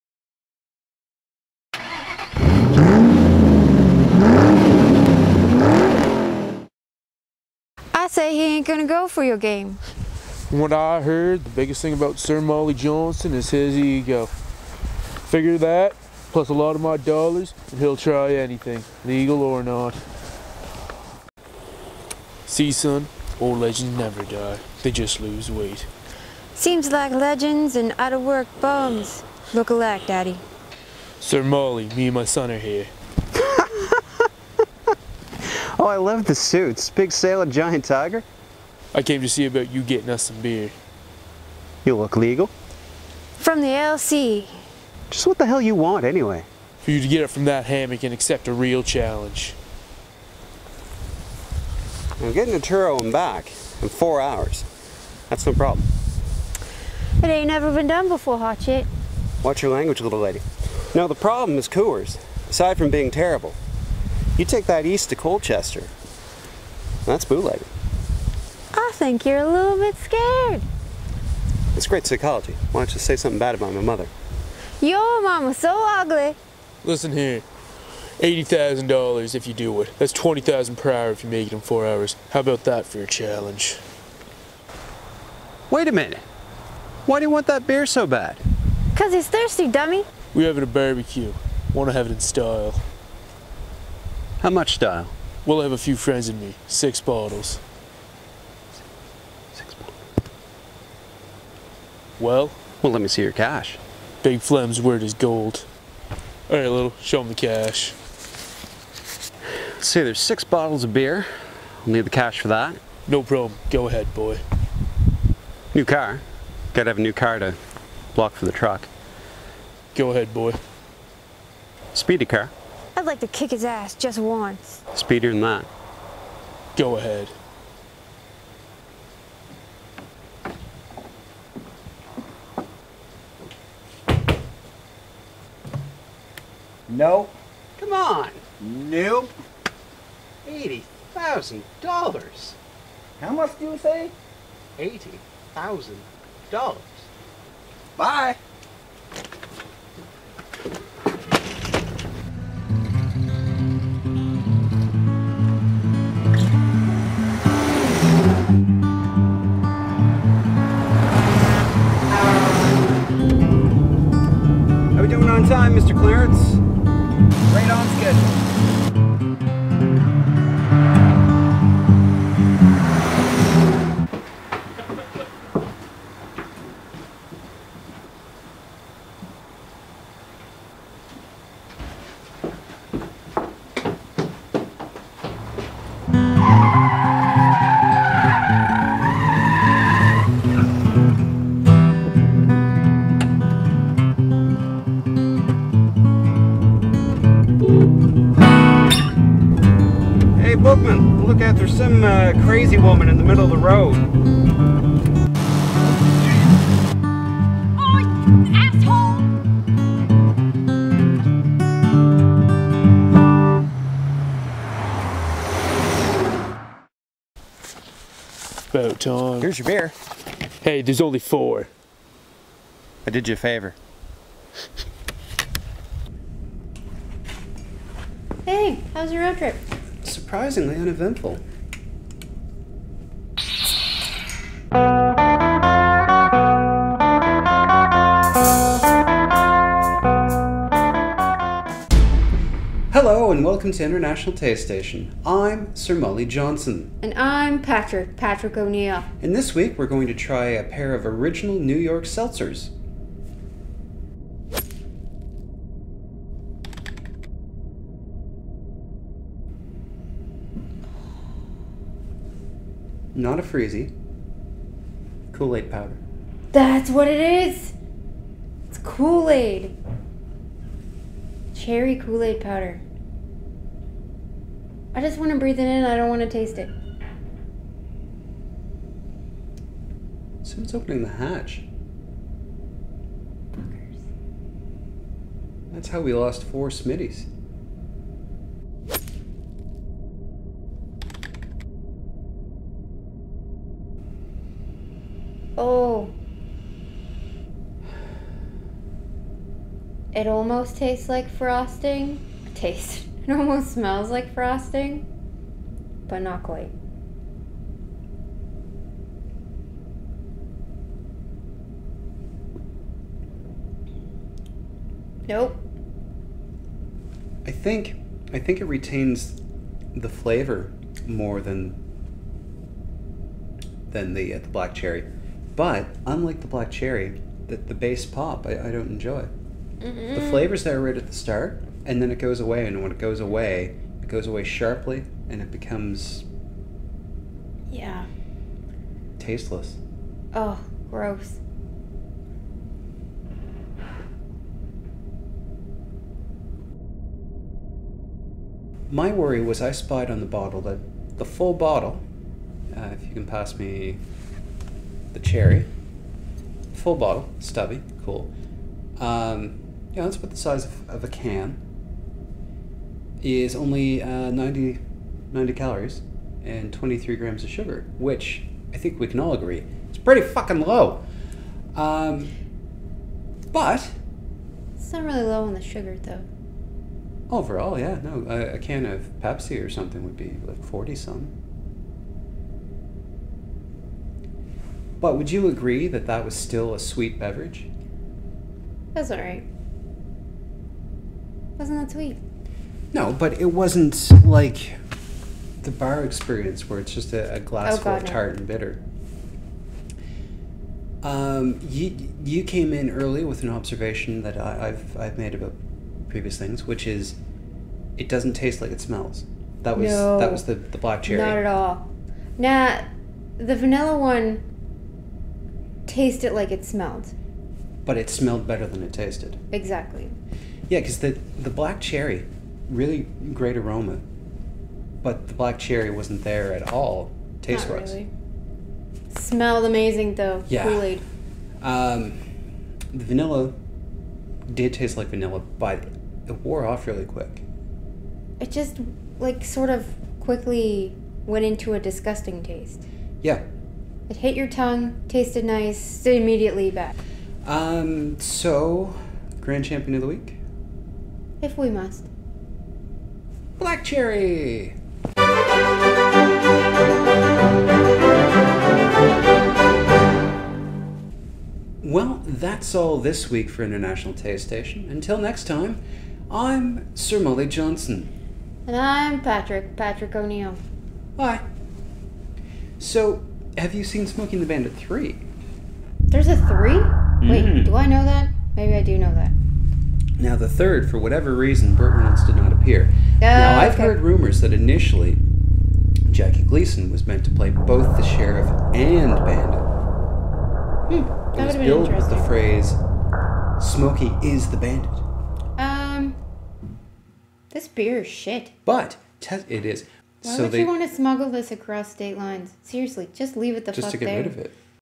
Mother Truckers/New York Seltzers | International Taste Station | Ep. 51
International Taste Station - The Internet's #1 Show Set in Space about Drinking Things.
Episode 51, wherein we have a long way to go and short time to get there.
Featuring:
Original New York Seltzer.

This was not a paid review.